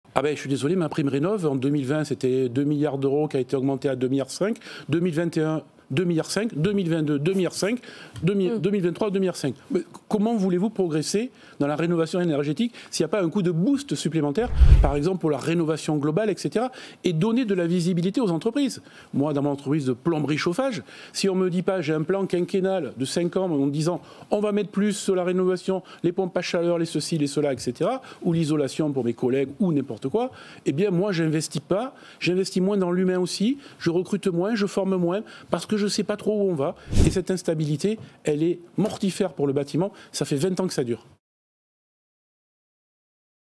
Rénovation énergétique: le coup de gueule de la Fédération du Bâtiment 🏢
"Comment voulez-vous progresser dans la rénovation énergétique s'il n'y a pas un coup de boost supplémentaire?"

Le coup de gueule d'Olivier Salleron, président de la Fédération française du bâtiment, qui estime que l'État n'a pas les moyens de ses ambitions